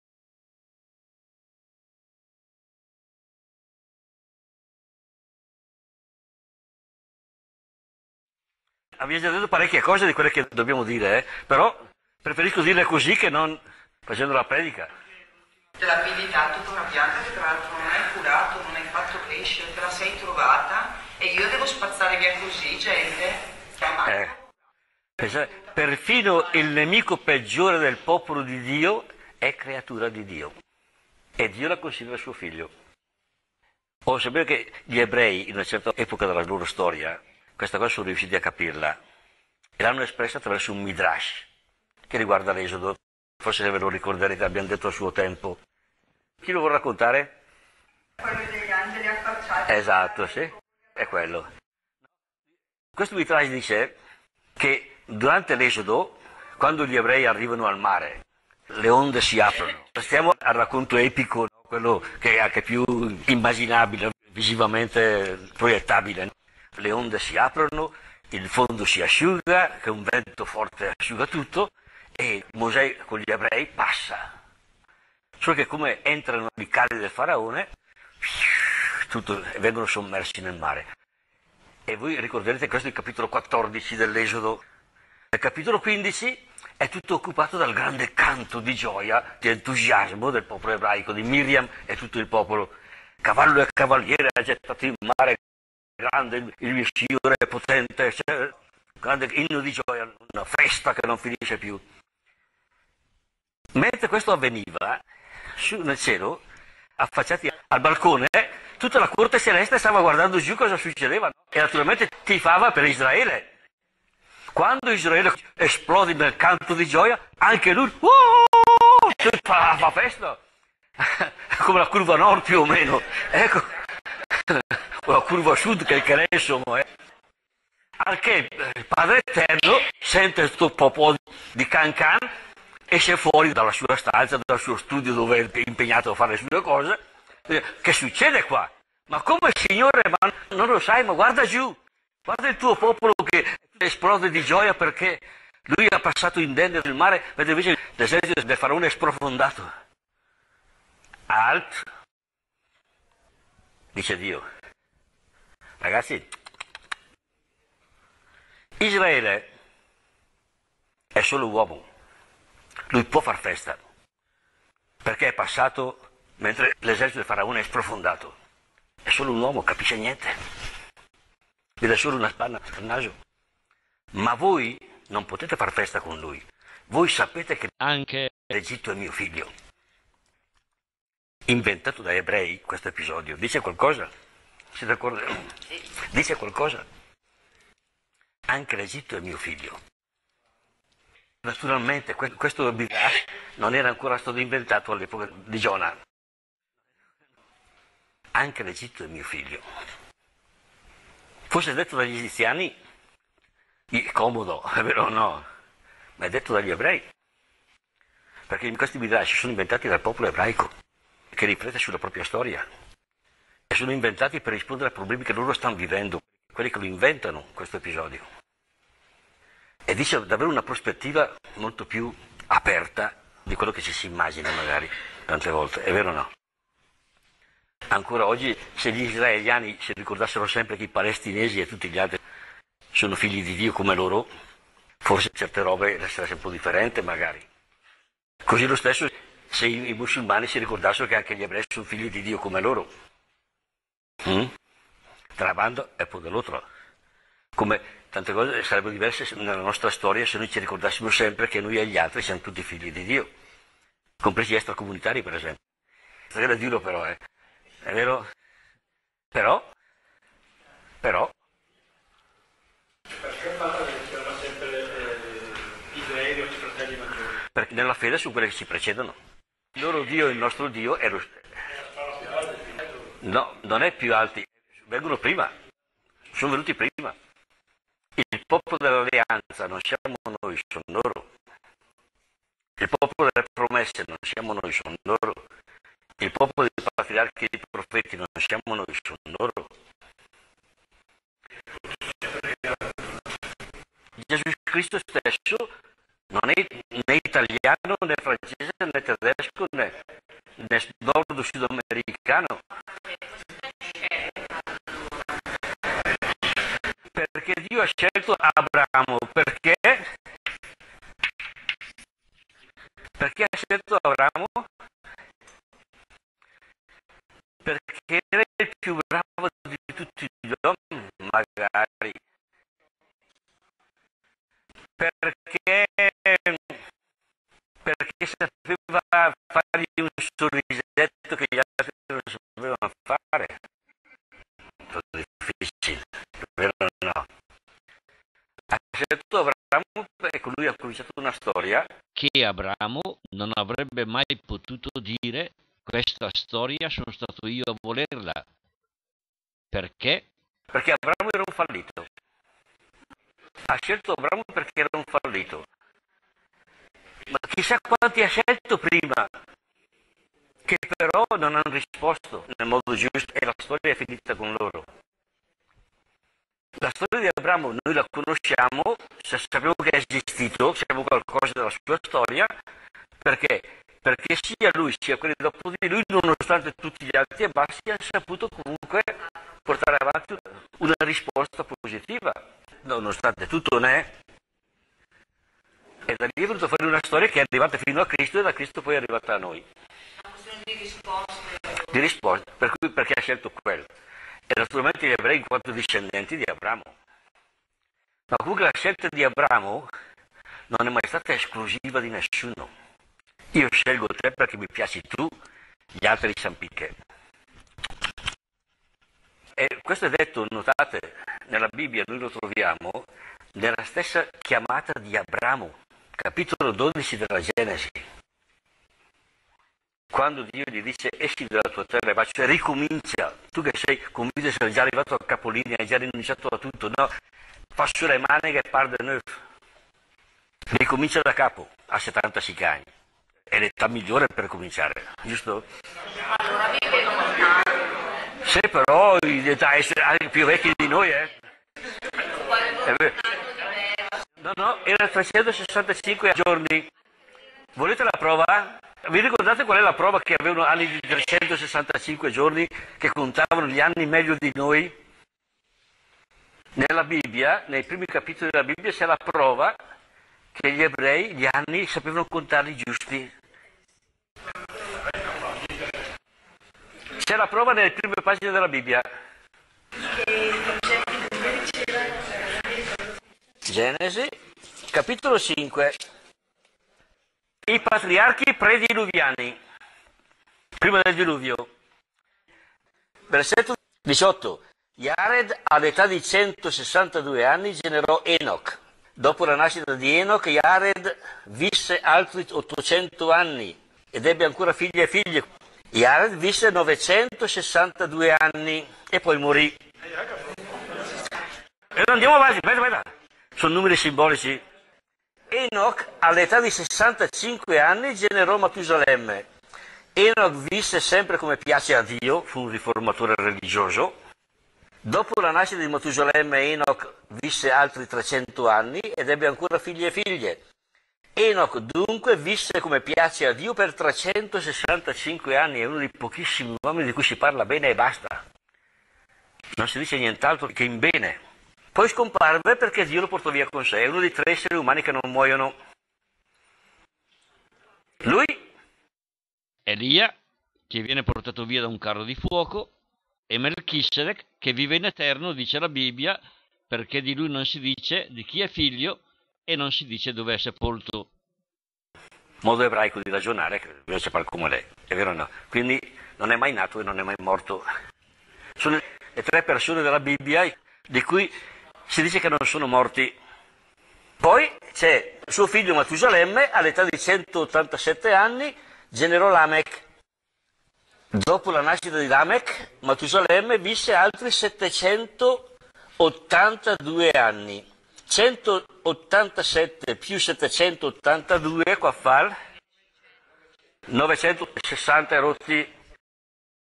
Giona (A08) - Anche un nemico di Dio è figlio di Dio?
La Bibbia in parrocchia - Don Pierino Ongaretti
Anche un nemico di Dio è figlio di Dio?
Ma perché Dio ha scelto Abramo e non...?